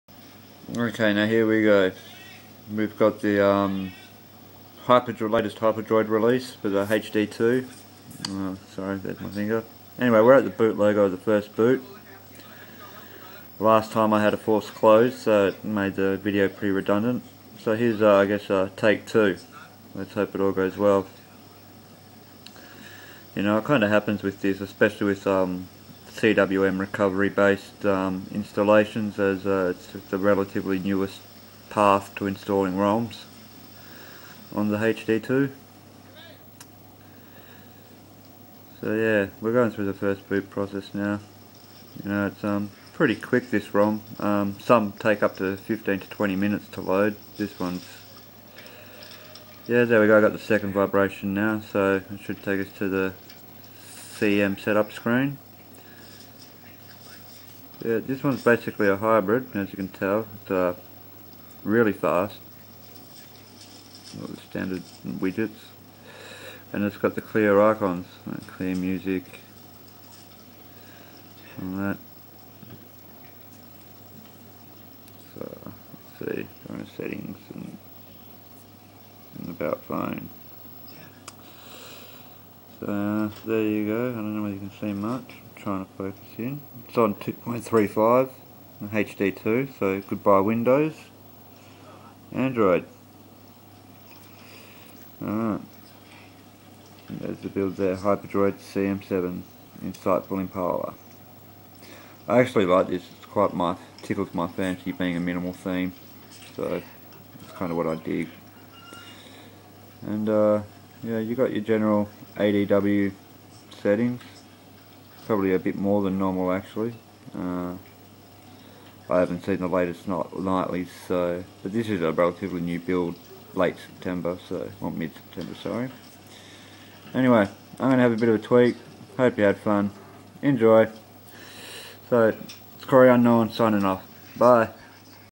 Htc hd2 running Hyperdroid rom
A quick video to show the average boot time from a cwm/magdlr android install with sept Hyperdroid release
***Trenchcoat Flashers Group***